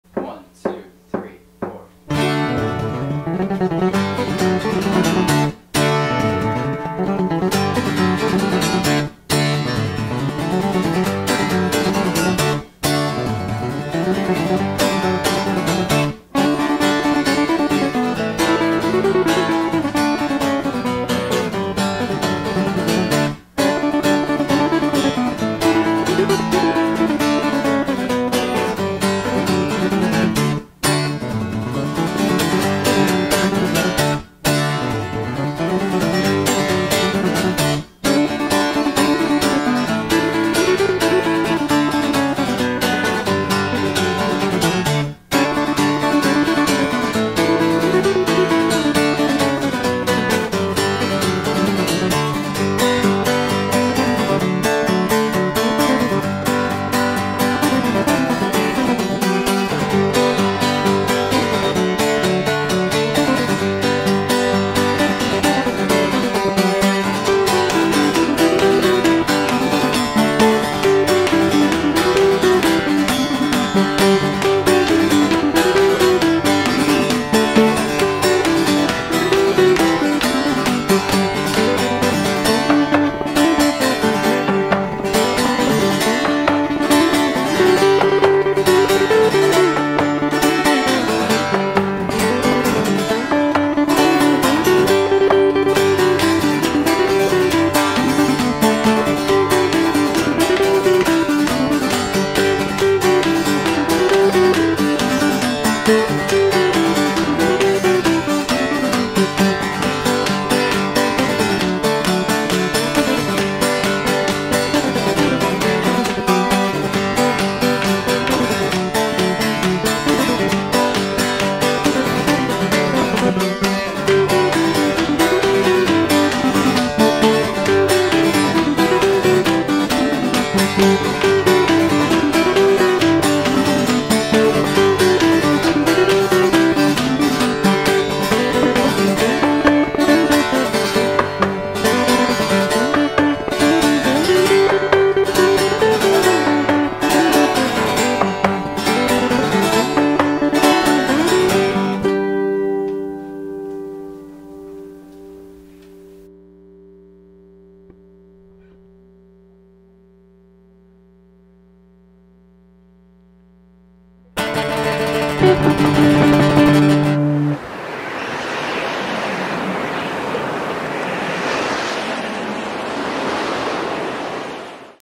Babin Brothers - 2018 - St Patrick's Day
Sam Babin - Lead guitar
Jon Babin - Rhythm guitar
Video - Nicholas Lager